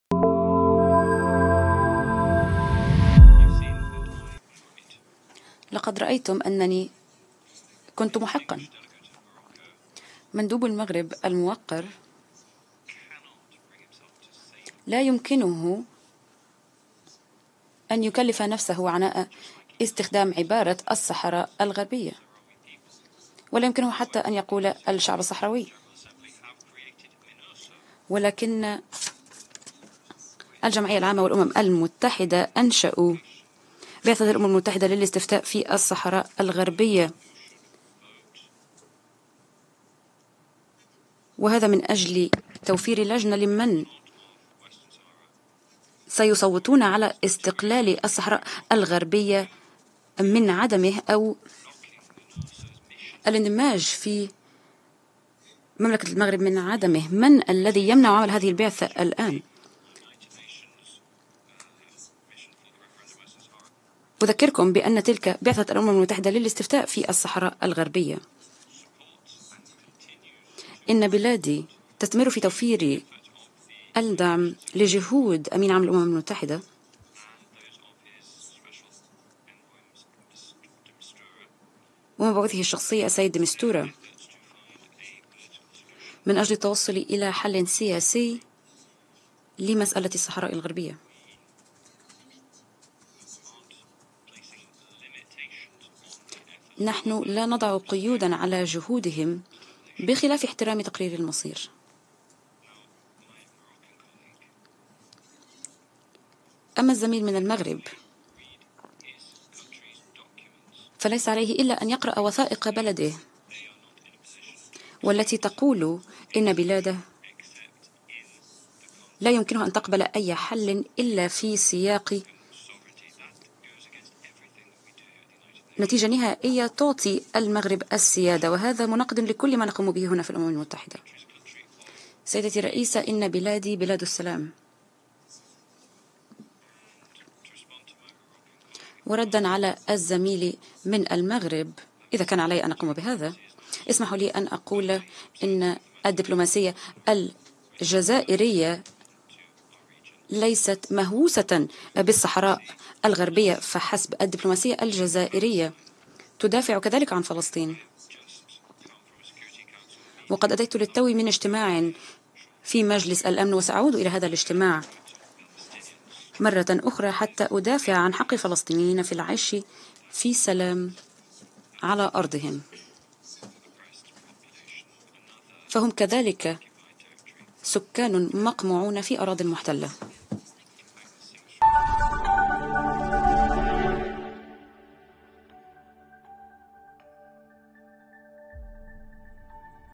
نيويورك: بن جامع يفكك بالادلة حجج ممثل المغرب في الدفاع عن أطروحاته الاستعمارية للصحراء الغربية
نيويورك(وأج)- خلال أعمال اللجنة الرابعة للأمم المتحدة المكلفة بإنهاء الاستعمار، فكك الممثل الدائم للجزائر السفير عمار بن جامع، حجج  ممثل المغرب للدفاع عن أطروحاته الاستعمارية للصحراء الغربية. وحاول ممثل المغرب الترويج لفكرة مضللة مضمونها  أن "مشاريع التنمية" في الأراضي المحتلة للصحراء الغربية تتماشى مع "ميثاق المستقبل". الذي يعتبر اعلانا سياسيا اعتمده رؤساء الدول والحكومات خلال أعمال الدورة 79 للجمعية العامة للأمم المتحدة شهر سبتمبر الماضي